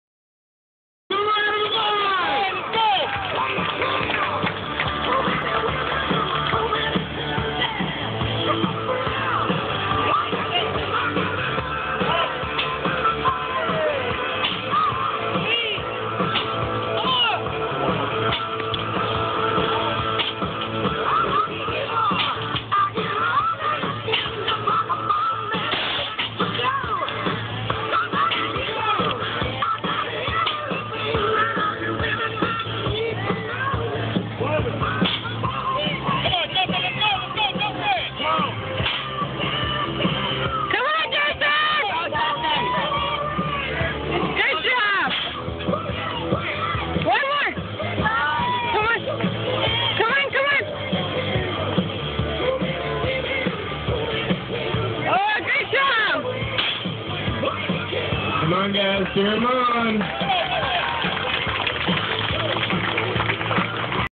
New Mexico NAS Competition
Pulling a heavy apparatus in New Mexico for Reps